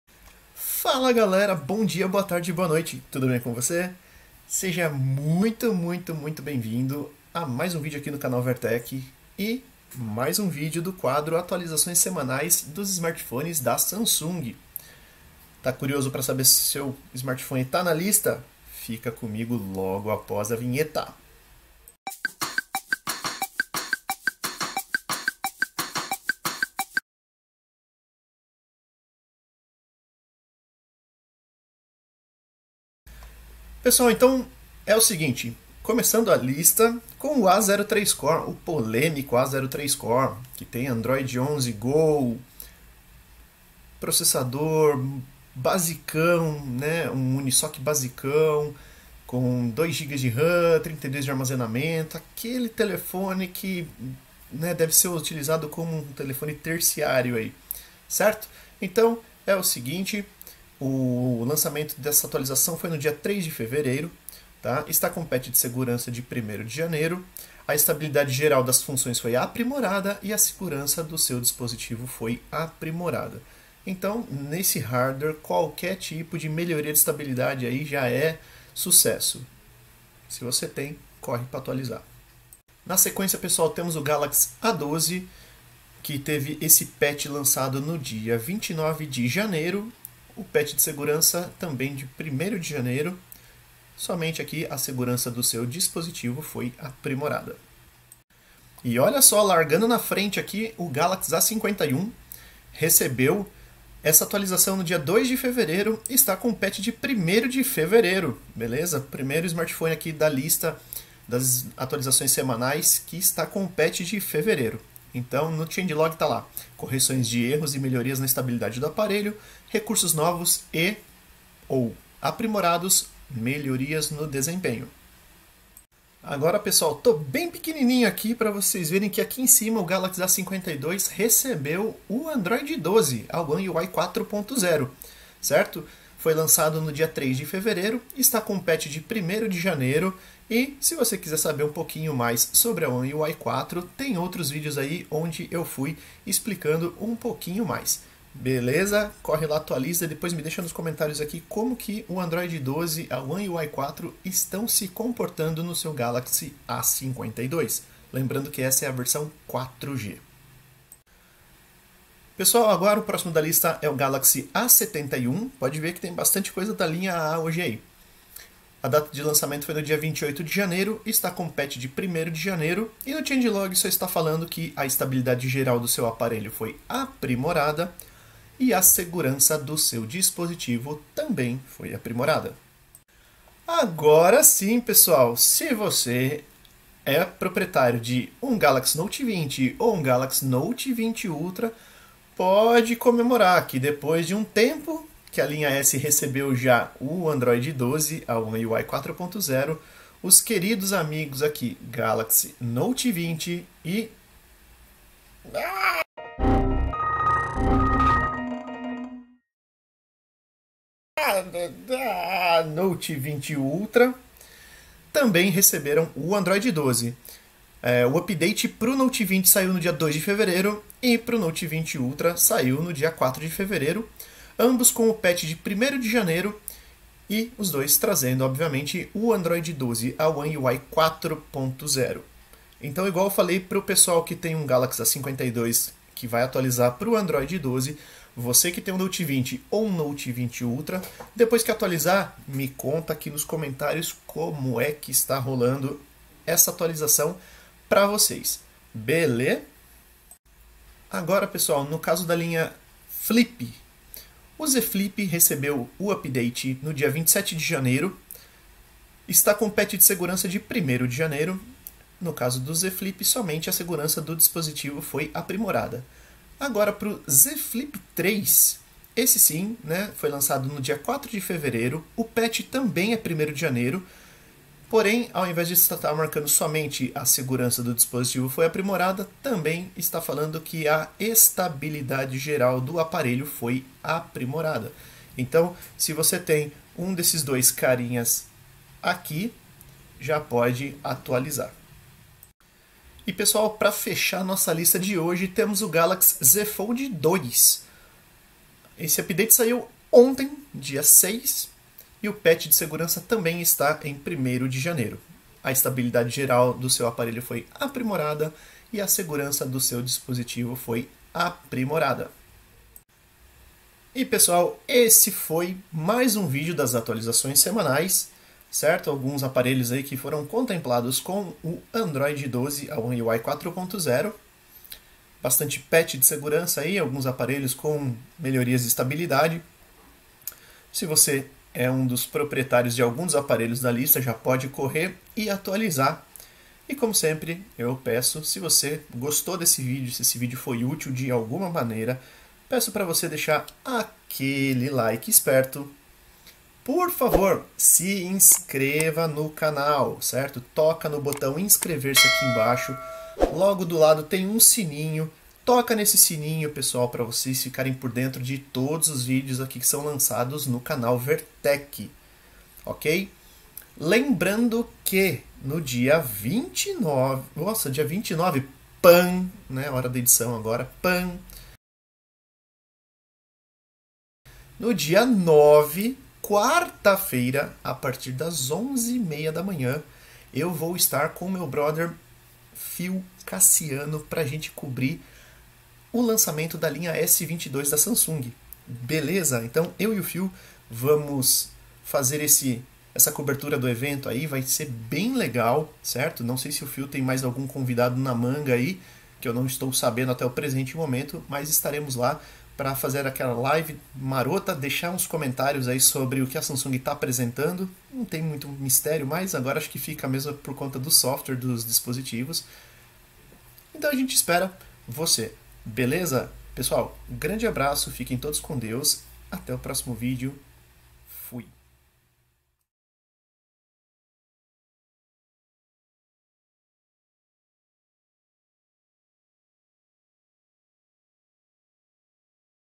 Atualizações Semanais Samsung #24
Bom dia, boa tarde e boa noite!

E no vídeo de hoje, trago pra vocês mais um episódio do quadro "Atualizações Semanais" dos smartphones da Samsung!

Canal do Phill pra vocês acompanharem a live de lançamento do dia 09/02!